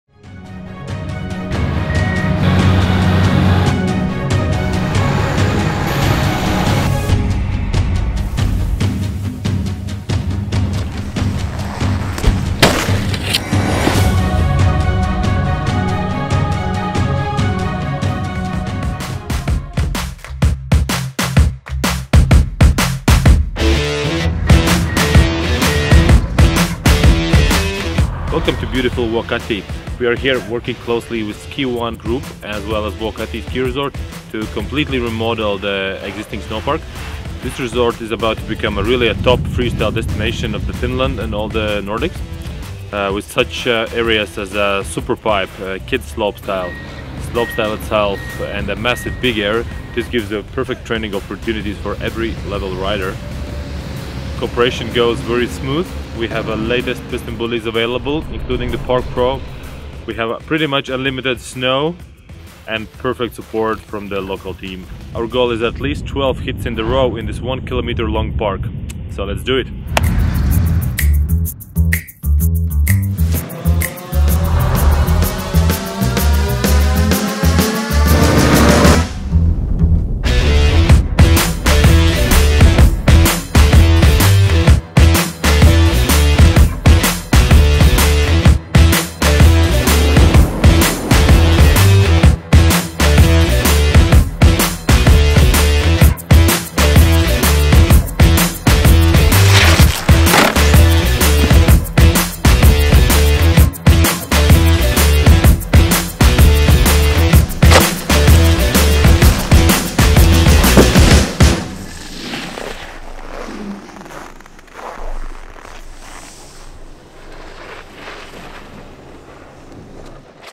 PistenBully x Schneestern | Vuokatti Snowpark, Finland | 2019
Built by Bully! Vuokatti Snowpark in Finland has been shaped up by the Baltic Schneestern Crew. It was their first project in Finland and the outcomes are amazing: 11 rad features, including a Big Air Jump are awaiting freeskiers and snowboarders from now on. We're proud to support our partner Schneestern in projects like this and thus, contribute to worldwide snowpark construction.